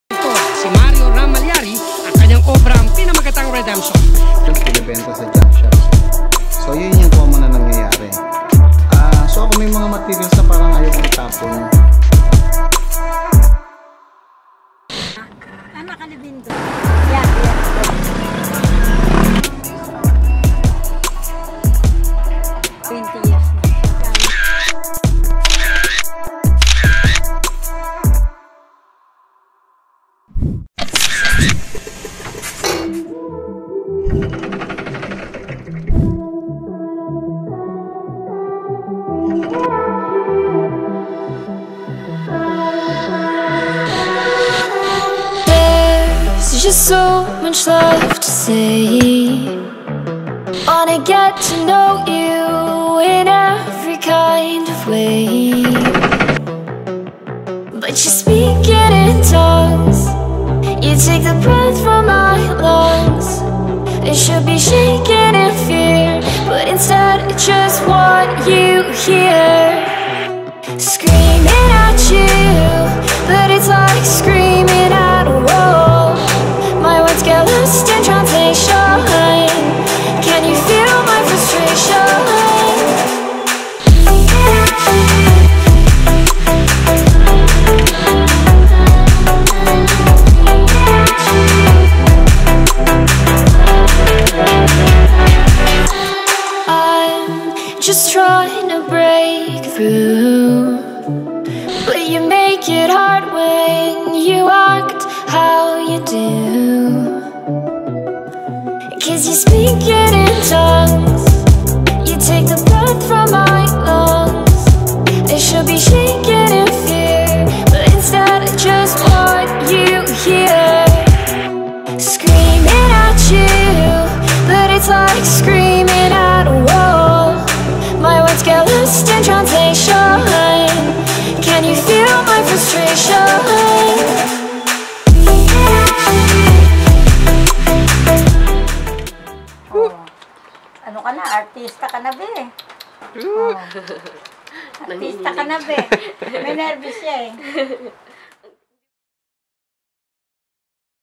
Steampunk By Mr. Ram Mallari Jr. | Vash Eliakim Films
Last May 22, 2019, we were invited to visit and to make a film of a gallery of an internationally acclaimed metal sculptor Mr. Ram Mallari Jr.

Some of his famous creations are located in Nuvali and other galleries in the Philippines.

I have met RogerRaker that day which is the grandson of Ms. Nieves Avendaño, the reason why we are there in the first place .
She is someone who reached out on me after seeing one of my posts in facebook.

This might be one of the most memorable thing that I've experienced because all of this started just because a random person got inspired by my post.

This was a blessing that I have never imagined.

Cinematography: Vash Eliakim Buenaflor